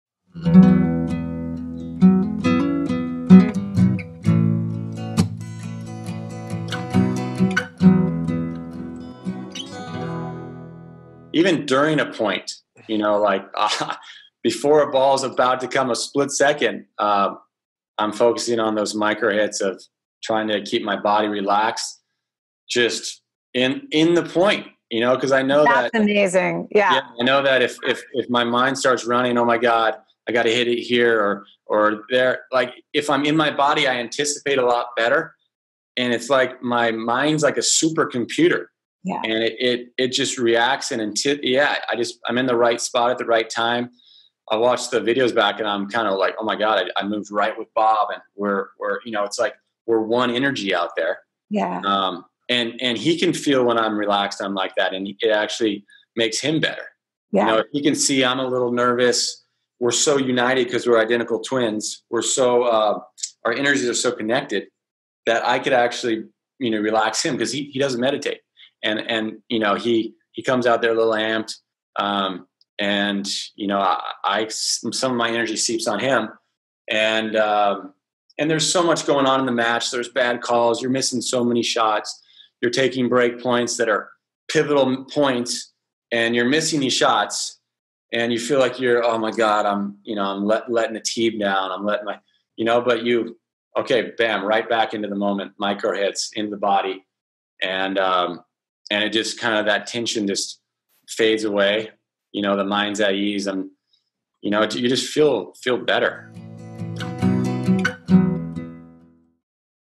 Mike Bryan - Meditation and Tennis / Maintaining Cool in a Match
Immersion 2020 - May 13th -17th, 2020

A 100% Free, 5 Day Mindfulness Summit and Live, Virtual Retreat.